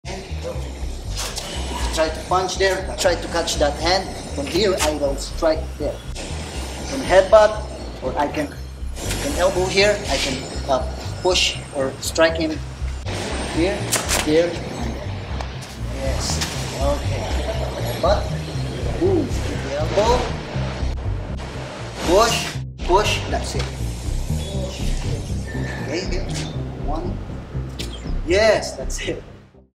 Filipino Dirty Boxing or Panantukan, in Manila
This is part of a class by Master Ronnie Royce (Laraw Kali Pamuok) when he visited our hotel in Manila to teach some Filipino boxing that is unorthodox and basically has no rules. Known as Panantukan and often referred to by Filipino teachers as ‘Mano-mano’ because it is really a fighting style learned on the streets of the Philippines.